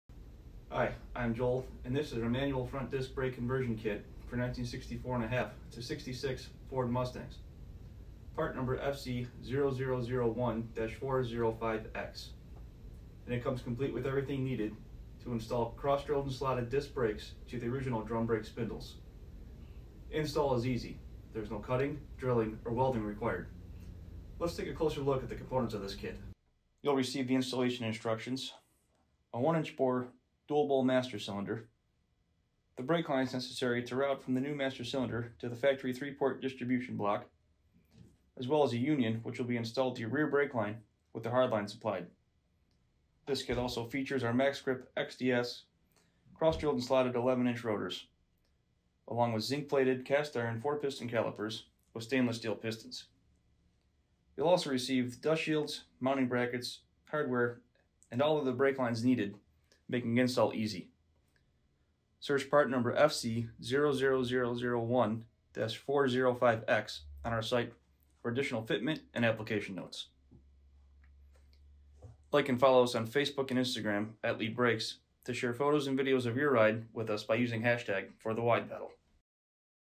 Manual Front Disc Brake Conversion Kit for 64.5-66 Ford Mustang with our MaxGrip XDS Rotors
Watch this short video to see the contents of our Manual Front Disc Brake Conversion Kit for 64.5-66 Ford Mustang with our MaxGrip XDS Rotors.